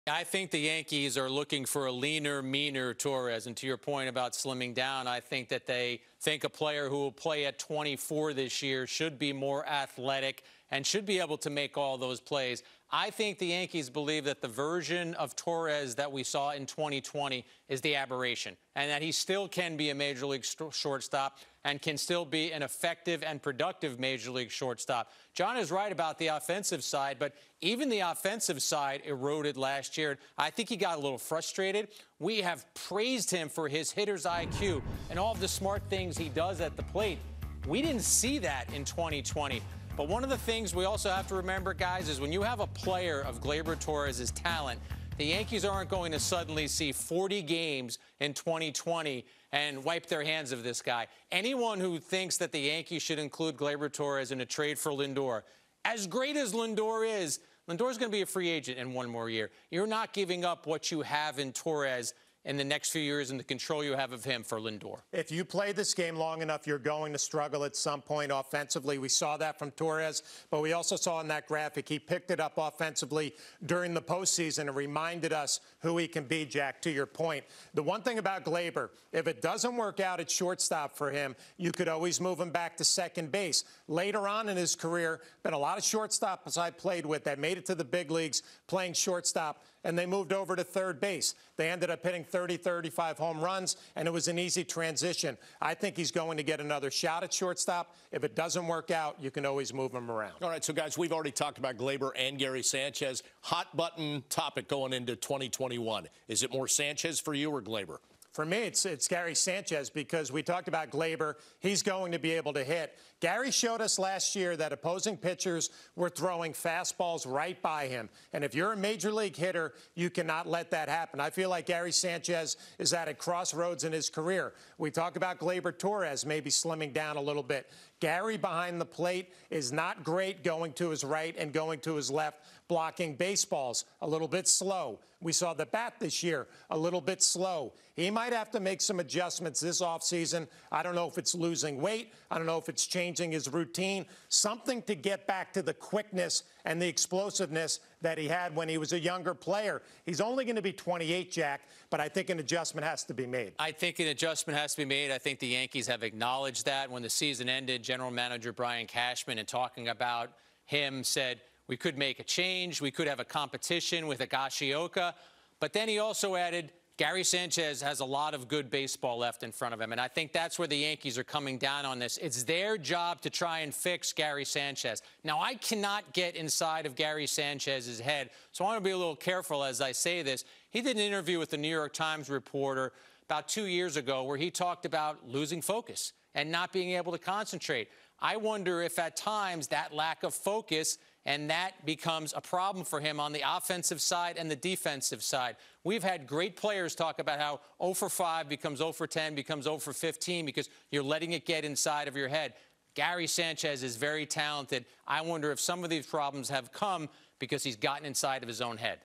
Gleyber Torres and Gary Sanchez's futures in pinstripes
The Yankees have some critical decisions when it comes to the roles of Gary Sanchez and Gleyber Torres next season in the Bronx.